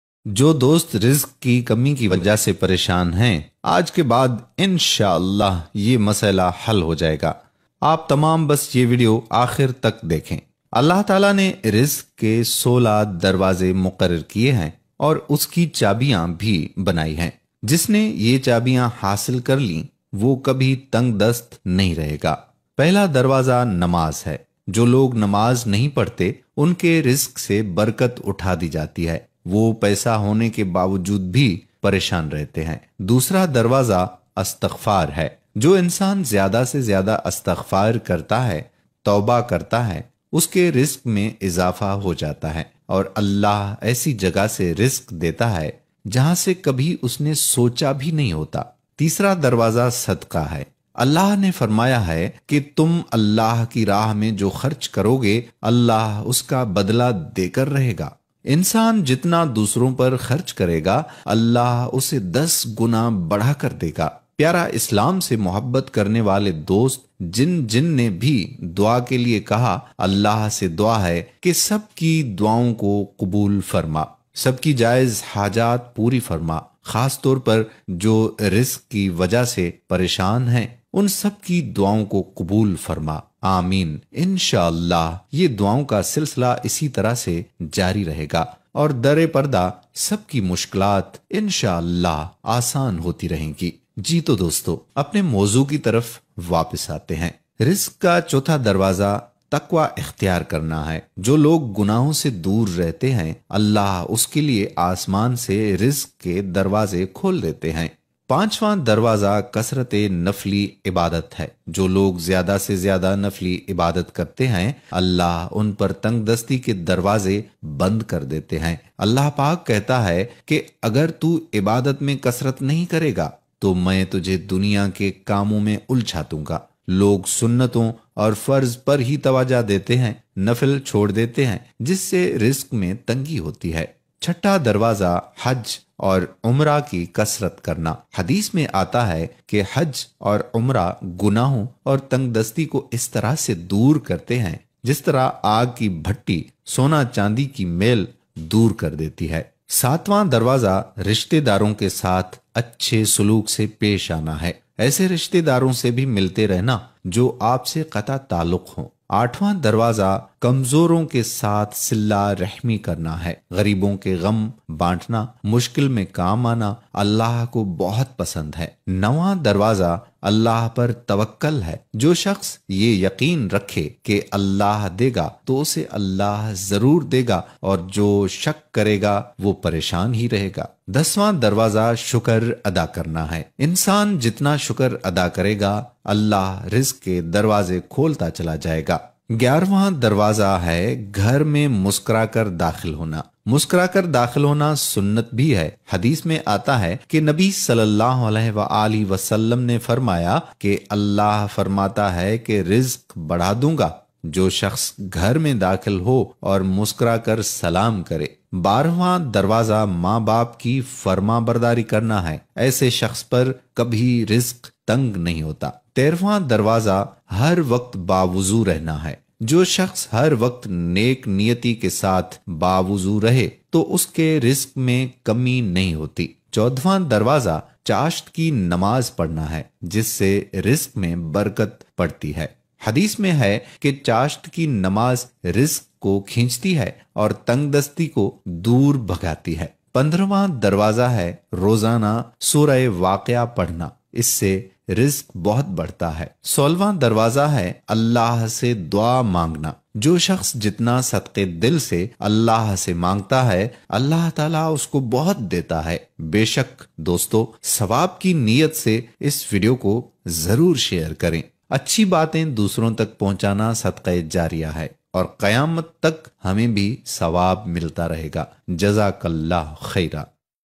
Rizq ke 16 Darwazy | Islamic Story "Hussain"
Dosto aaj iss video me aap janege rizq ke 16 darwaze ke bare me ki kaun kaun 16 darwazy rizq ke hai or isase kya fyada hai
rizq ke 16 darwazy aur unki chabyan

***Declaimer***
By Copyright

Within 24hrs!